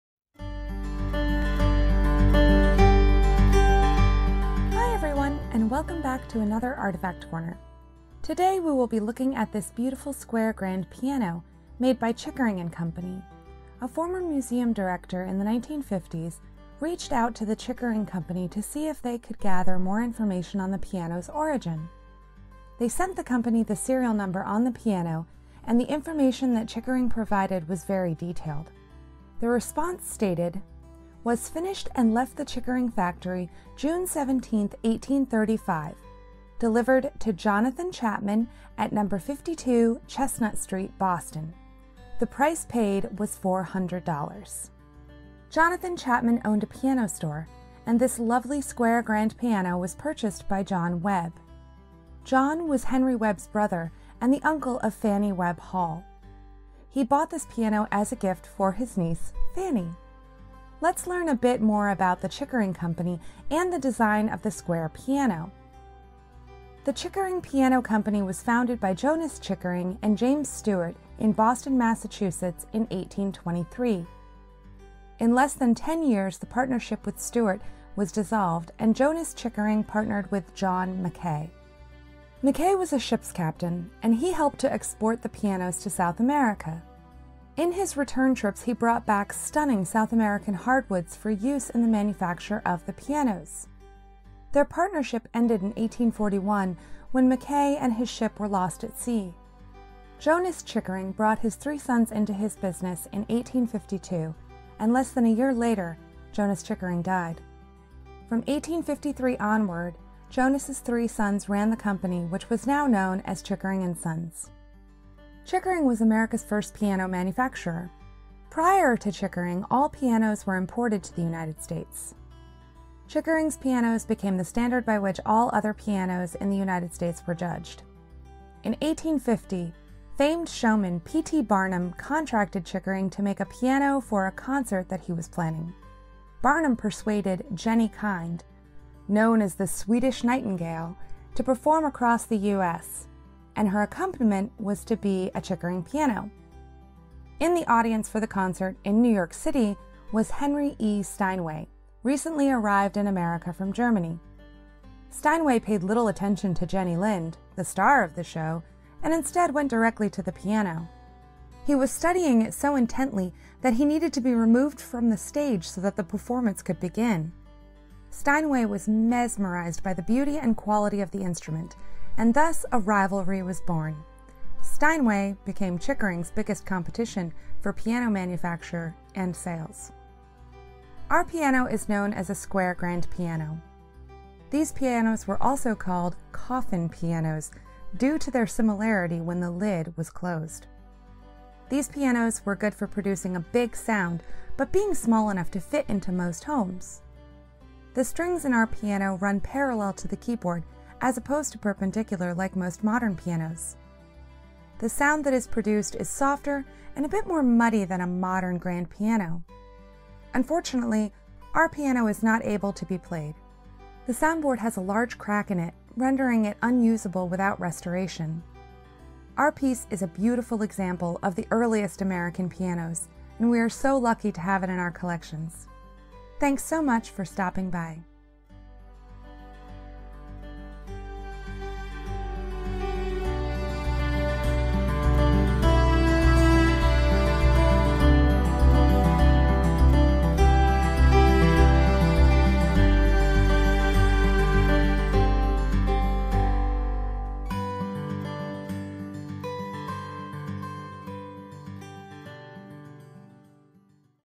KDHM Artifact Corner: Episode 35 - Chickering Piano
Today, we have a musical artifact to examine.

Hi Everyone and welcome back to another artifact corner. Today we will be looking at this beautiful square grand piano, made by Chickering & Co. A former museum director in the 1950’s reached out to the Chickering Company to see if they could gather more information on the piano’s origin. They sent the company the serial number on the piano, and the information that Chickering provided was very detailed. The response stated, “was finished and left the Chickering Factory June 17th, 1835, delivered to Jonathan Chapman at #52 Chestnut Street, Boston. The price paid was $400.00.” Jonathan Chapman owned a piano store, and this lovely square grand piano was purchased by John Webb. John was Henry Webb’s brother and the Uncle of Fannie Webb Hall. He bought this piano as a gift for his niece, Fannie. Let’s learn a bit more about the Chickering Company, and the design of the square piano.
The Chickering piano company was founded by Jonas Chickering and James Stewart in Boston, Massachusetts in 1823. In less than 10 years the partnership with Stewart was dissolved, and Jonas Chickering partnered with John Mackay. Mackay was a ships captain, and he helped to export their pianos to South America. In his return trips he brought back stunning South American hardwoods for use in the manufacture of the pianos. Their partnership ended in 1841 when Mackay and his ship were lost at sea. Jonas Chickering brought his three sons into his business in 1852, and less than a year later, Jonas Chickering died. From 1853 onward Jonas’ three sons ran the company which was now known as Chickering and Sons.
Chickering was America’s first piano manufacturer. Prior to Chickering, all piano’s were imported to the United States. Chickering’s pianos became the standard by which all other pianos made in the United States were judged against. In 1850, famed showman P.T. Barnum contracted Chickering to make a piano for a concert that he was planning. Barnum persuaded Jenny Kind, known as the Swedish Nightingale to preform across the US, and her accompaniment was to be a Chickering piano. In the audience for the concert in NYC was Henry E. Steinway, recently arrived in America from Germany. Steinway paid little attention to Jenny Lind, the star of the show, and instead went directly to the piano. He was studying it so intently that he needed to be removed from the stage so that the performance could begin. Steinway was mesmerized by the beauty and quality of the instrument, and thus a rivalry was born. Steinway became Chickering’s biggest competition for piano manufacture and sales.
Our piano is known as a square grand piano. These pianos were also called coffin pianos due to their similarity when the lid was closed. These pianos were good for producing a big sound, but being small enough to fit into most homes.The strings in our piano run parallel to the keyboard as opposed to perpendicular like most modern pianos. The sound that is produced is softer and a bit more muddy than a modern grand piano. Unfortunately, our piano is not able to be played. The soundboard has a large crack in it, rendering it unusable without restoration. Our piece is a beautiful example of the earliest American pianos, and we are so lucky to have it in our collections. Thanks so much for stopping by!